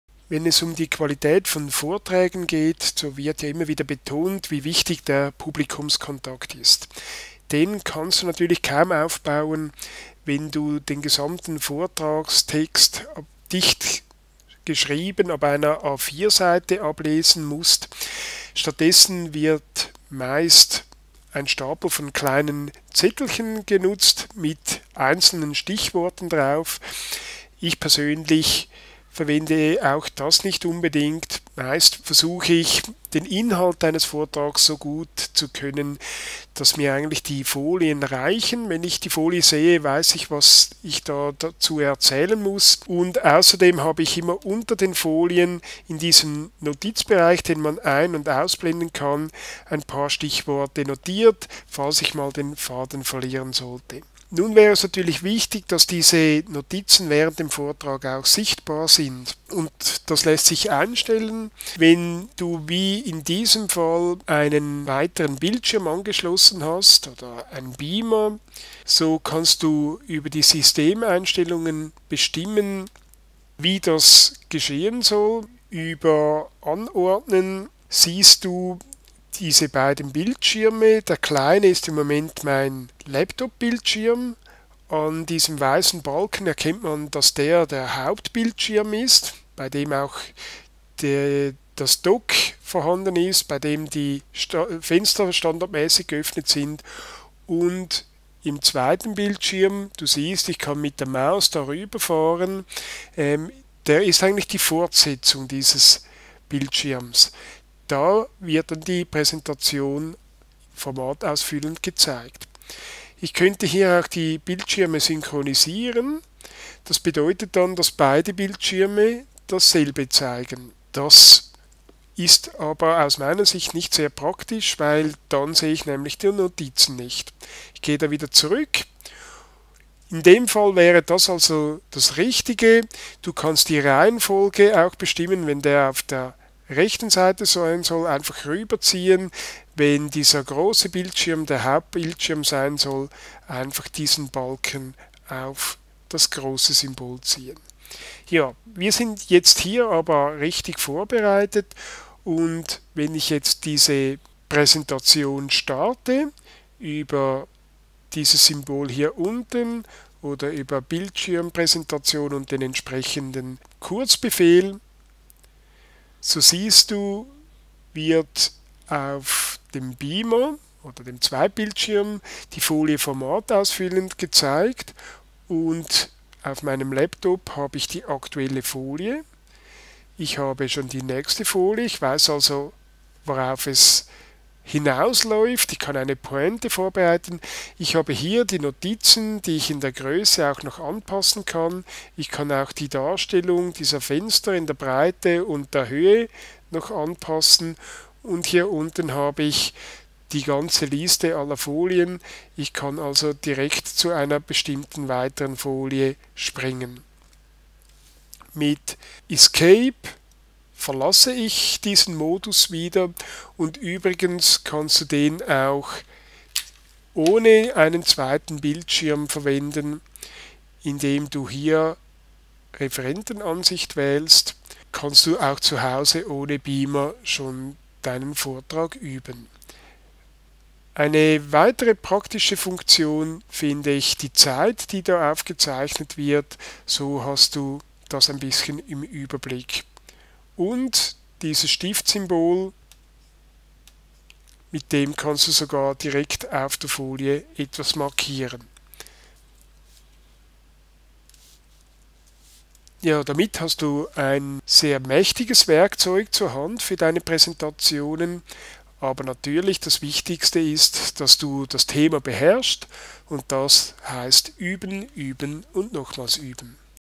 Powerpoint 2016 Mac: 31 Tipps zum Präsentieren mit Beamer
Notizen nutzen, Monitoreinstellungen am Mac, Referentenansicht anpassen